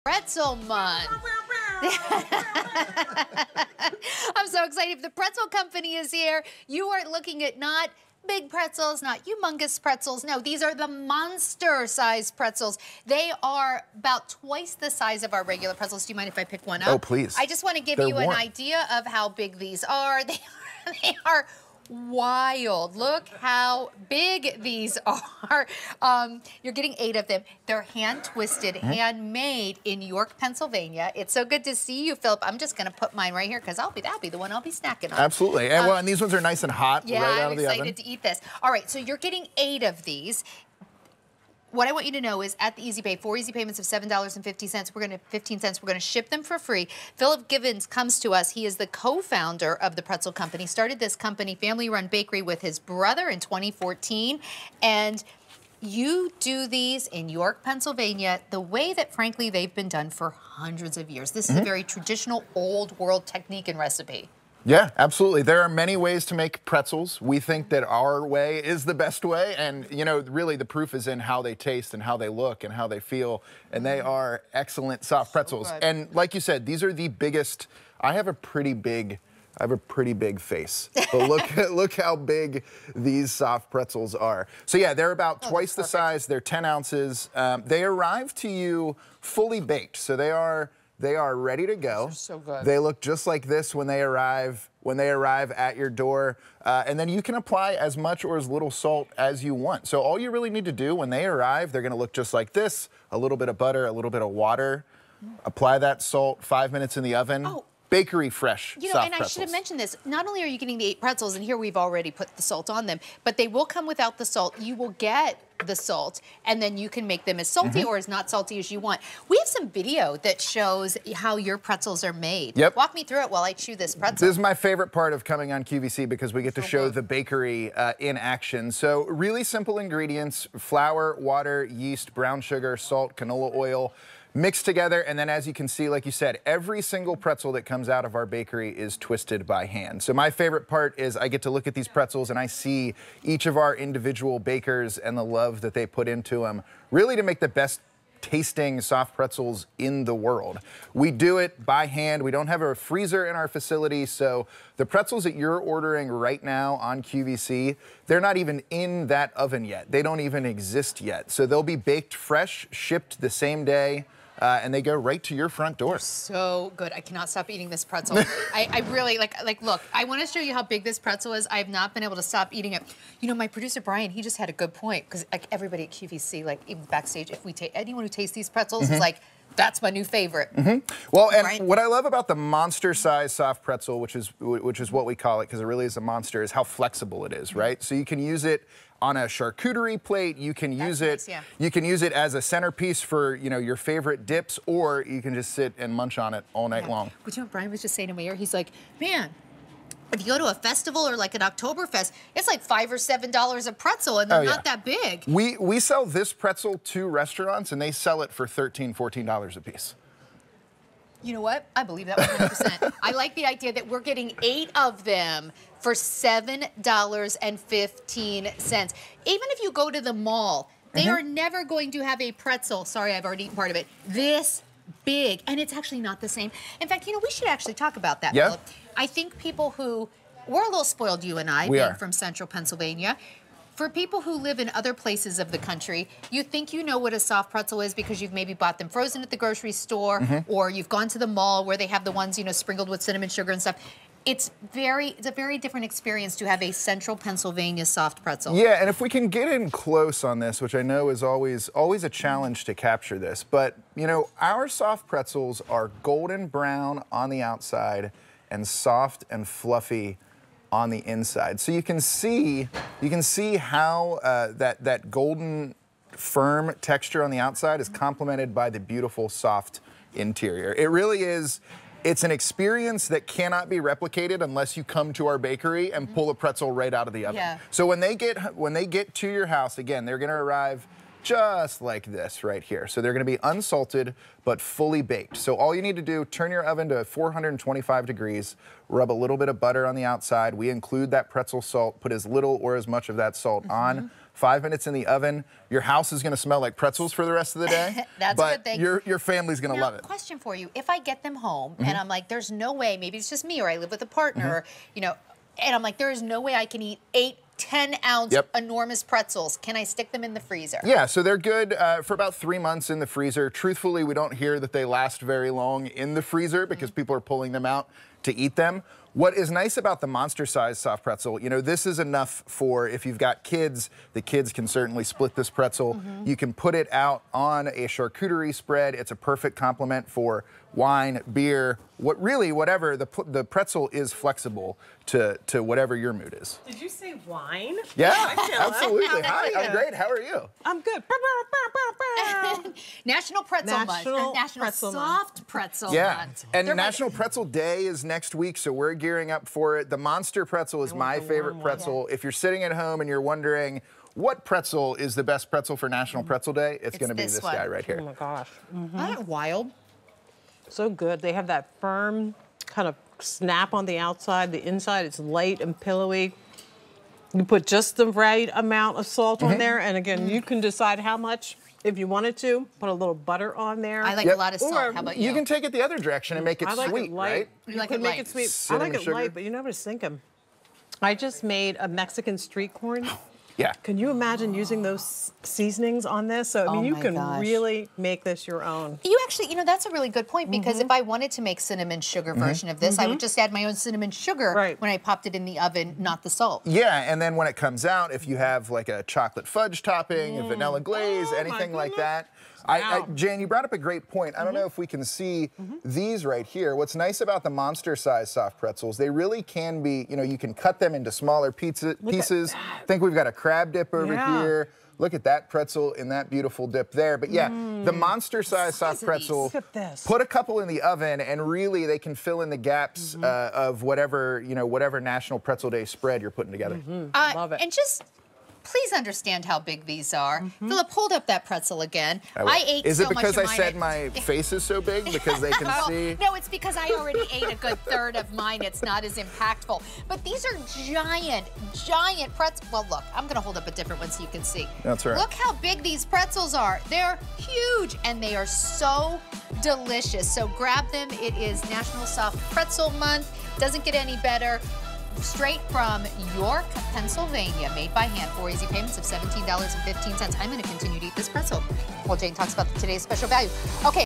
The Pretzel Co. (8) 10-oz Hand Twisted Monster Size Soft Pretzels on QVC
The Pretzel Co. (8) 10-oz Hand Twisted Monster Size Soft Pretzels

Made with just six ingredients (flour, water, salt, brown sugar, yeast, and canola oil) and baked fresh, these hand-twisted colossal-sized soft pretzels are soft, chewy, and so delish. From The Pretzel Co.